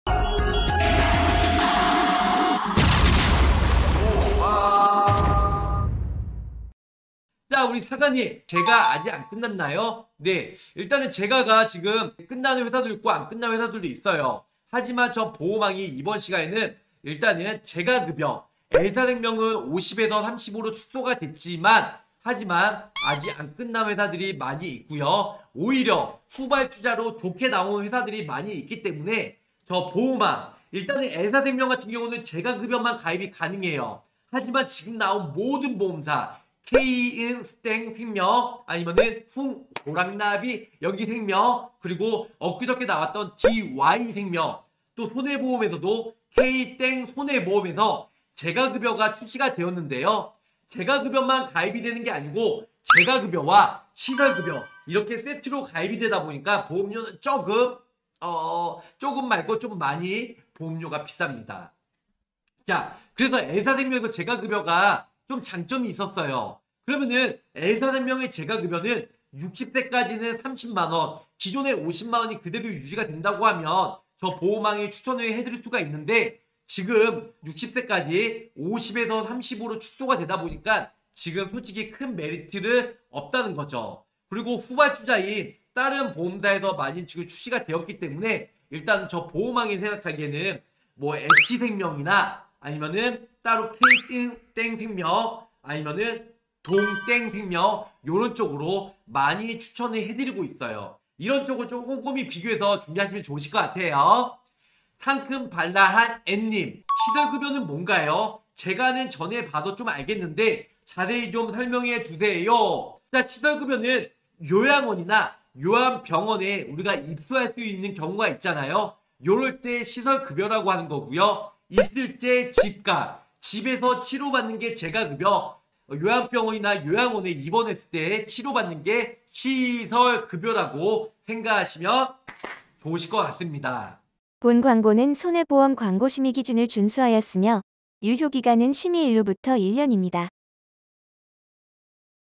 치매보험보다는 재가급여 시설급여!! (치매+상해+질병 몸이 불편하면?)
치매보험보다 재가 시설급여로 보장이
가능한 내용으로 설명드립니다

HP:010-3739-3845
카톡친구추가:r2an
카톡플러스친구:보험왕